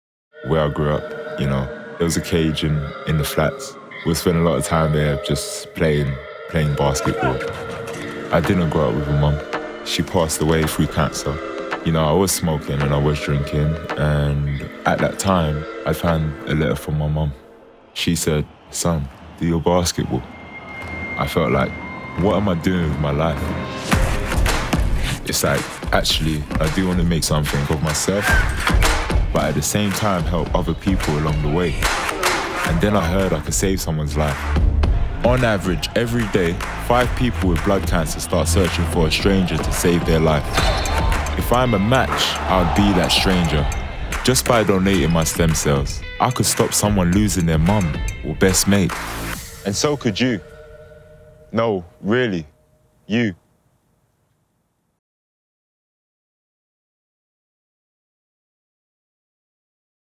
Lamar’s journey to becoming a potential stem cell donor
Lamar Roberts turned his life around and became a potential lifesaving stem cell donor in the process.